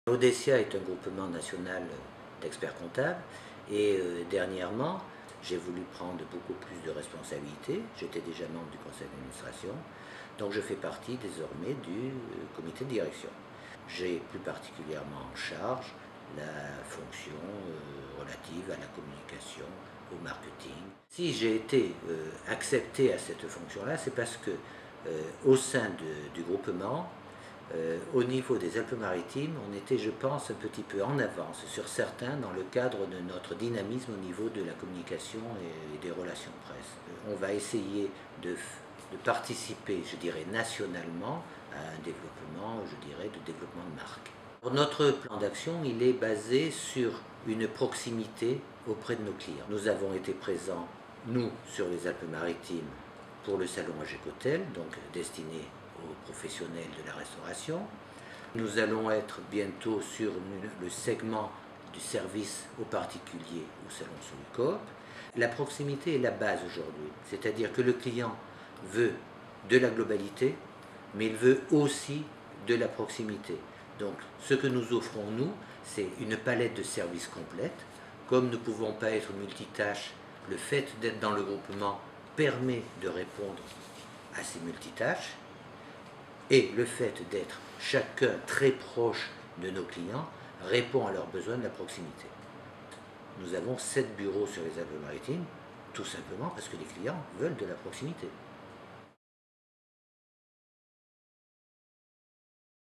Michel Cibelly élu vice-président d'Audecia
Le réseau national de cabinets d'experts-comptables a choisi son représentant niçois pour asseoir ses compétences en matière de communication, relations presse et marketing au niveau national. Isabelle Auzias. Complément du n°664 du Tribune-Bulletin Côte d'Azur